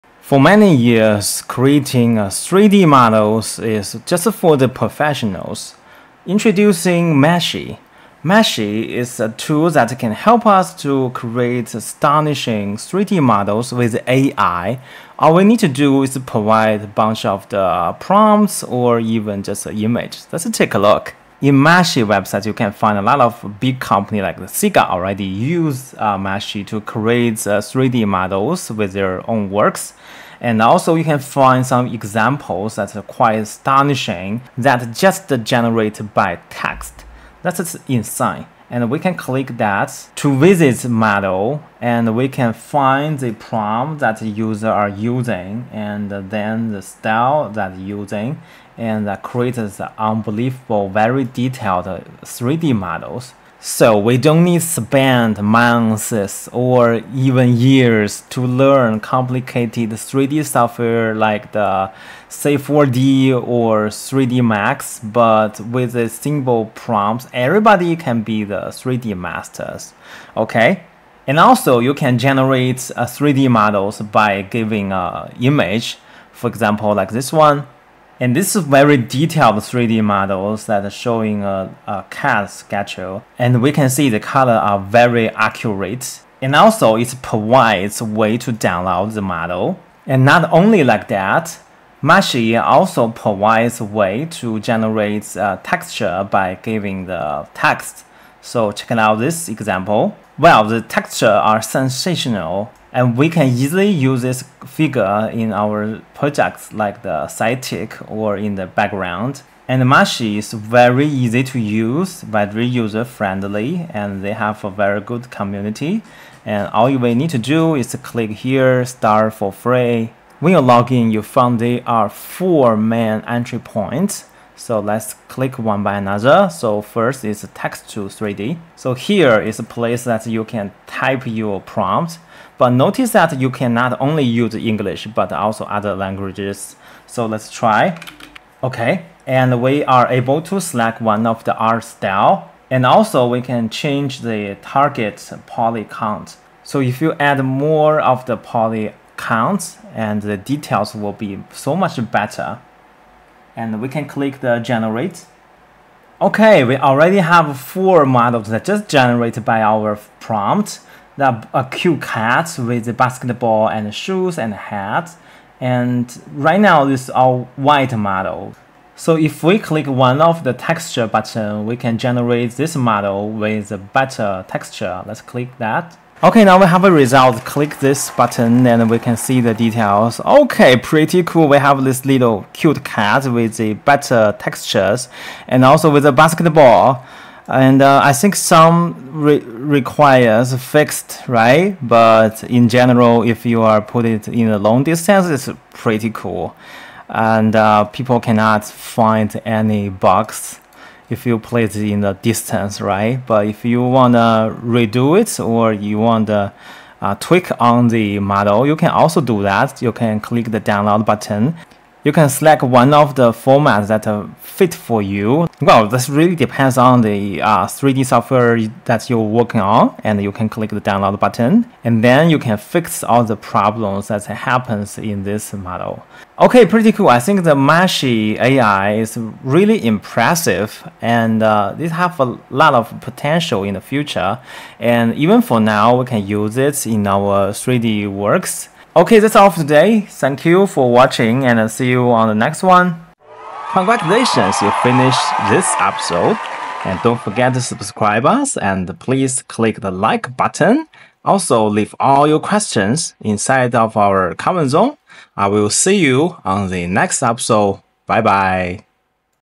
Introducing Meshy AI: Generate 3D Models with AI in Just a Minute
Meshy AI is an innovative tool designed to simplify 3D mesh generation and manipulation using the power of artificial intelligence. This AI-driven platform allows users to quickly create, edit, and optimize 3D models without needing extensive technical knowledge in 3D modeling. Whether you're a game developer, product designer, or someone exploring the world of 3D creation, Meshy AI streamlines the workflow by automating complex tasks like mesh refinement, texture application, and even animation preparation.

With its intuitive interface, Meshy AI empowers users to produce high-quality, detailed meshes suitable for a wide range of applications, from gaming and animation to virtual reality and product visualization. This makes it an ideal tool for both beginners and professionals looking for a more efficient way to handle 3D design. It leverages advanced machine learning algorithms to ensure that the generated meshes are optimized for performance, reducing file sizes without sacrificing detail.

UX Bootcamp, updating free design & AI tutorials for everyone!